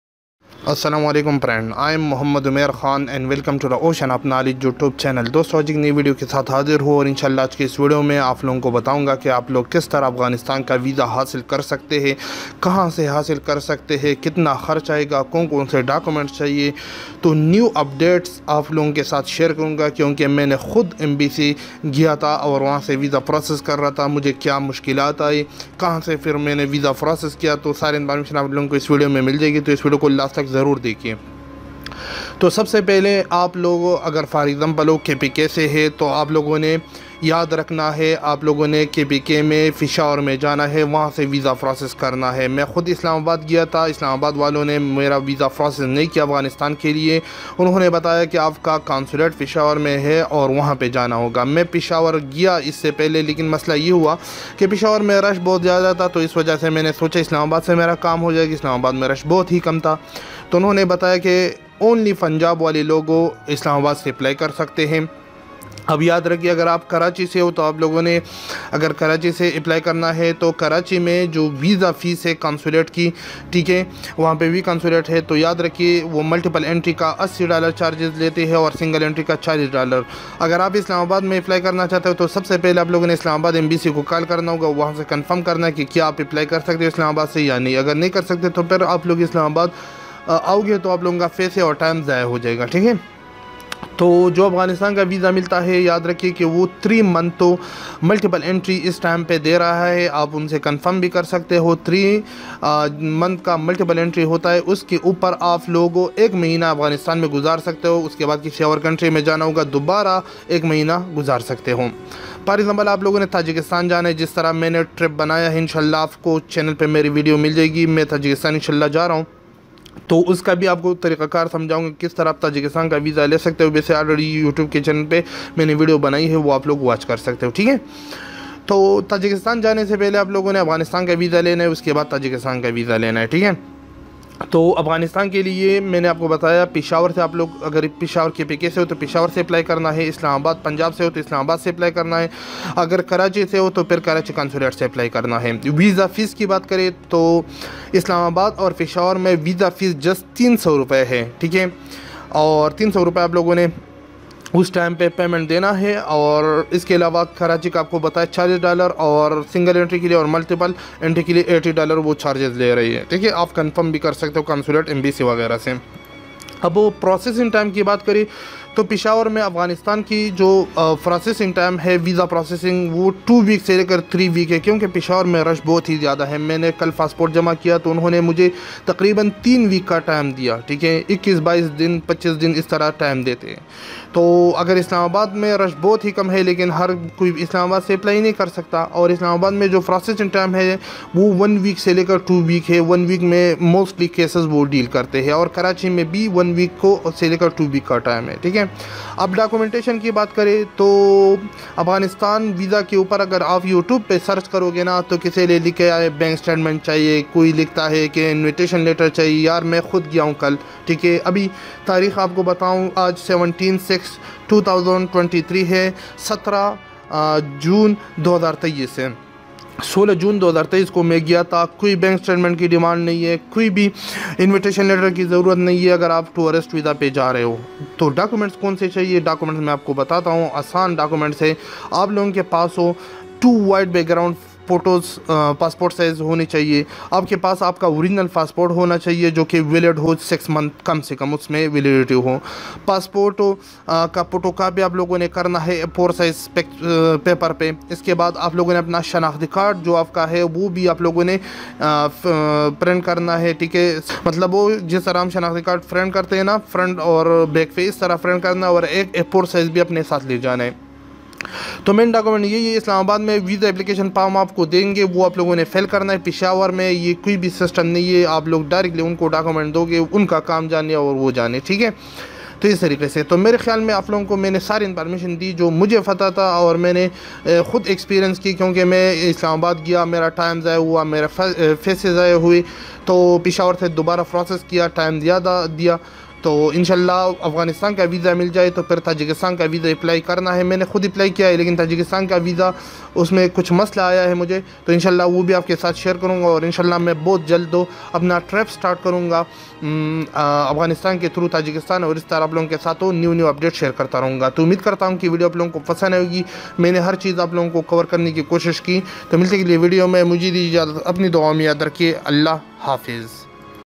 How To Apply And Get Afghanistan Tourist Visa | Afghanistan Tourist Visit Visa Documents Requirement
𝗔𝗯𝗼𝘂𝘁 𝘁𝗵𝗶𝘀 𝘃𝗶𝗱𝗲𝗼 :

In this video tutorial, you will learn about  How To Apply And Get Afghanistan Tourist Visit Visa And What Are The Main Documents Requirement For Afghanistan Visa.

Whatsapp Voice or Text Message Only: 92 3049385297 or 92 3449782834

                  __/ 𝗟𝗜𝗡𝗞𝗦 \__